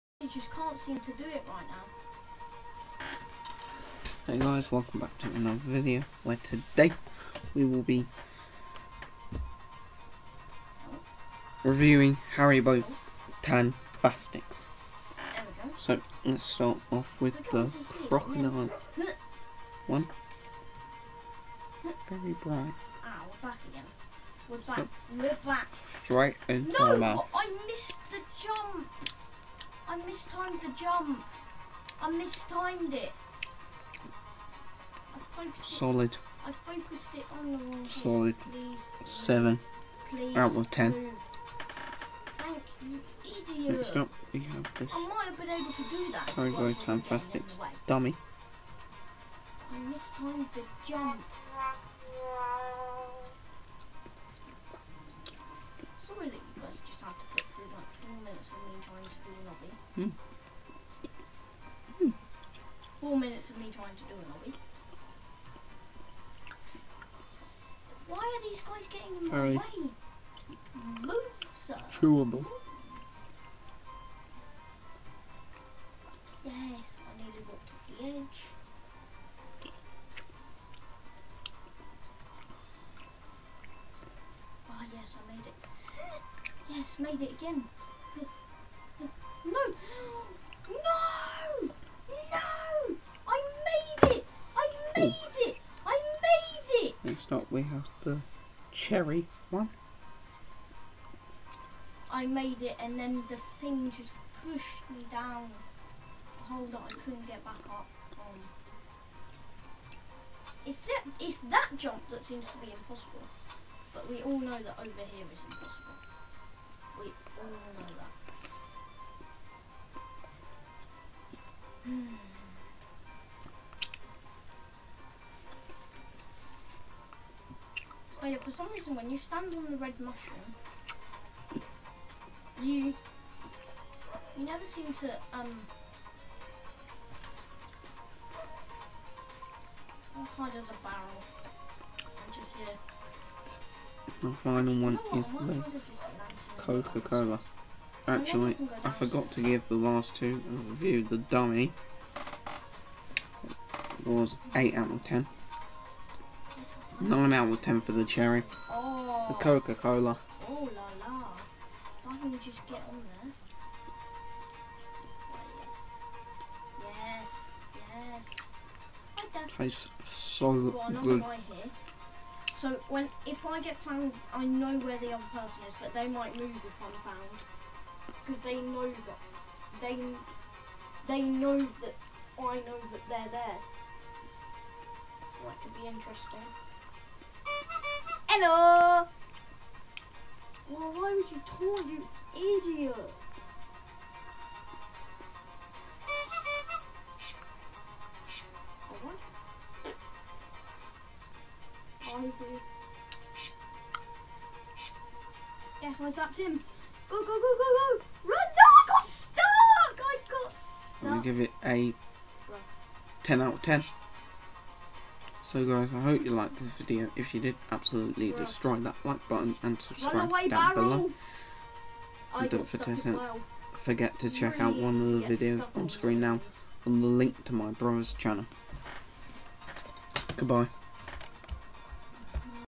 Reviewing Haribo Tangfastics
In this video I will be reviewing Haribo Tangfastics and remember this is my opinion cause I know you guys may have a different opinion to me